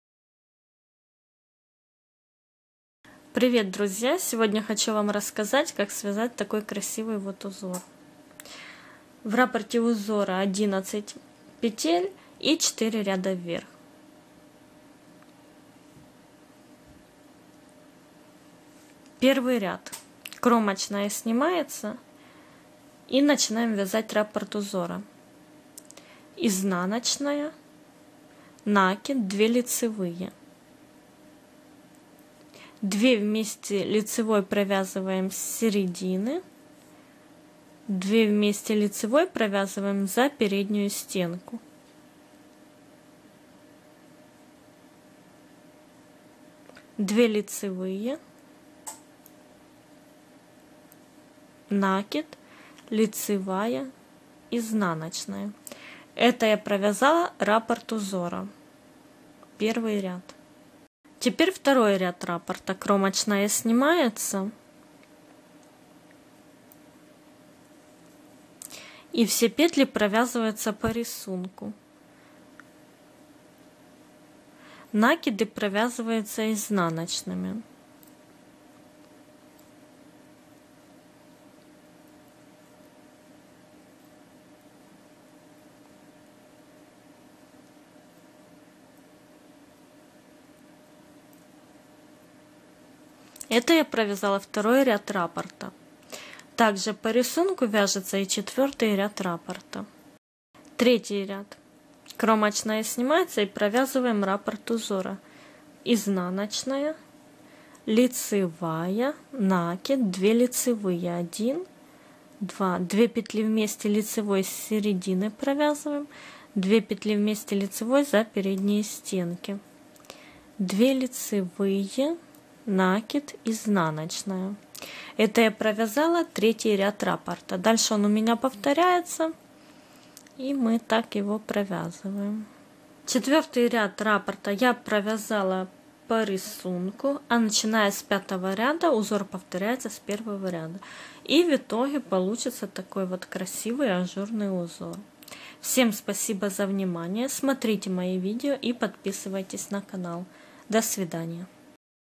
Красивый ажурный узор. Вязание спицами.
Красивый ажурный узор. Вязание спицами. Рапорт узора 11 петель и 4 ряда.